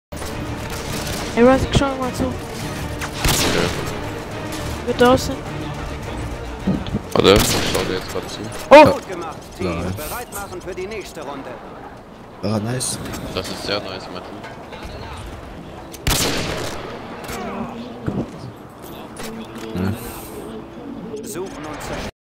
A4 Metal - Not sure #1
I've never done one use or lose episode, so I start now making those whenever I'm not sure about the clip is worth for people watching it in an episode or not.

The lobbys was lagging from the beginning on, yet I hit. The killcam is fucked up, so I'm not sure if I should use it (also since it's a standart spot).